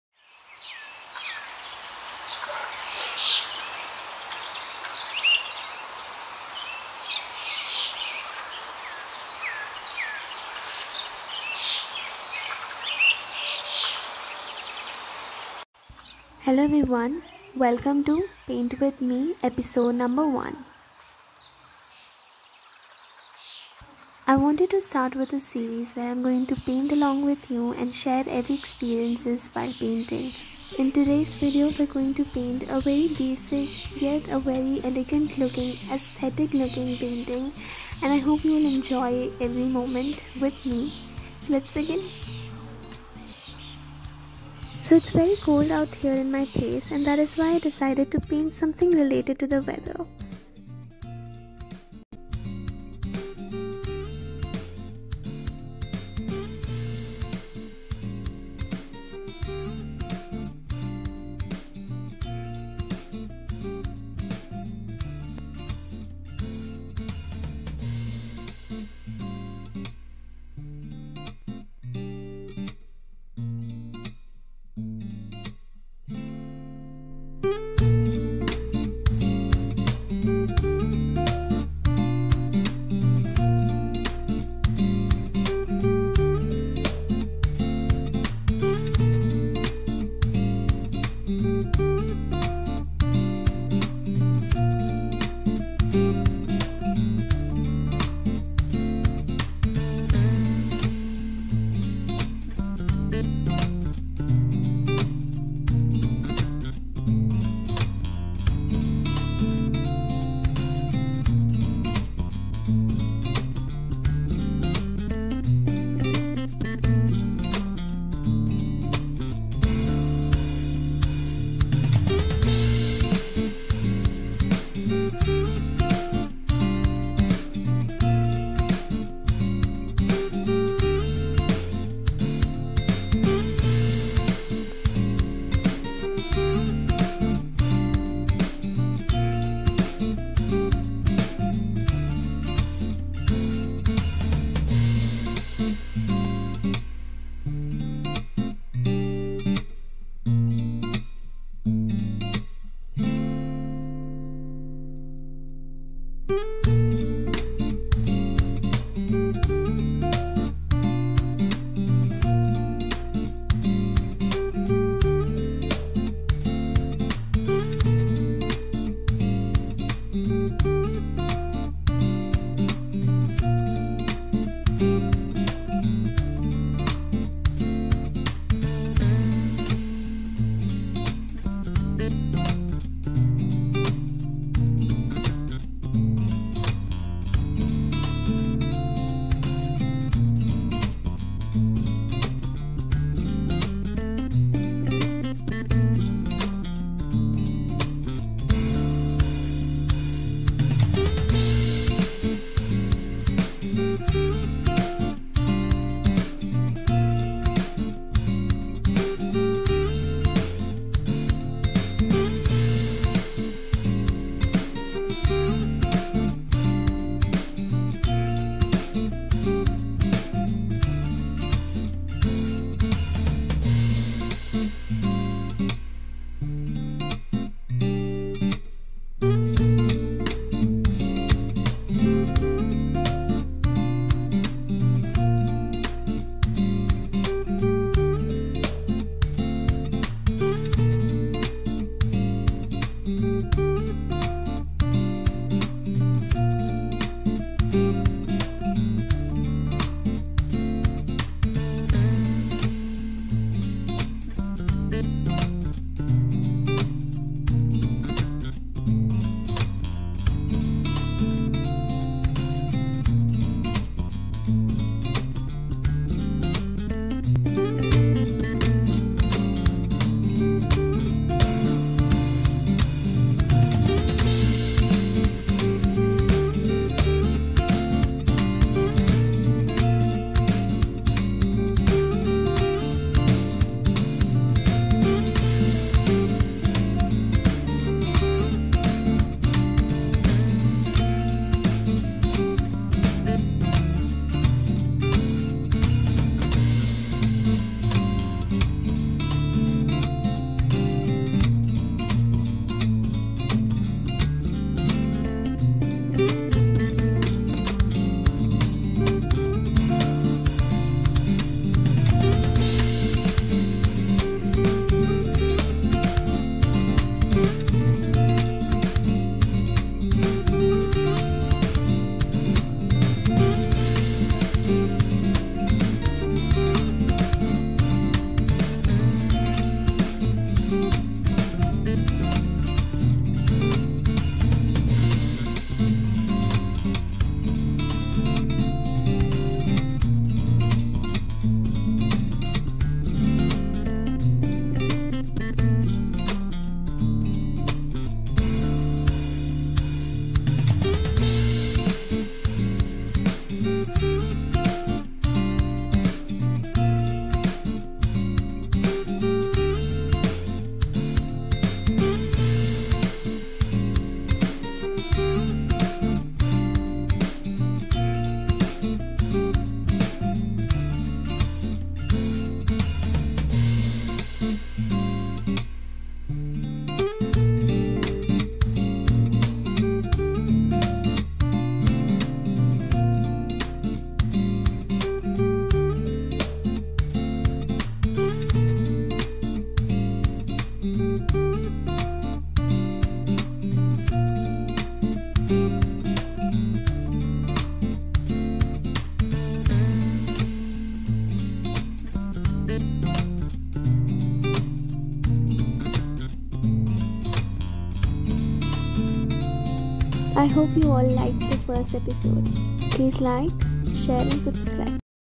Paint with me Ep-1|| galaxy watercolor painting||kritika's dance and art
hey guys welcome back to my channel I hope you like the episode one of my series and if yes then do wait  for the next episode till then like share and subscribe to my channel thank you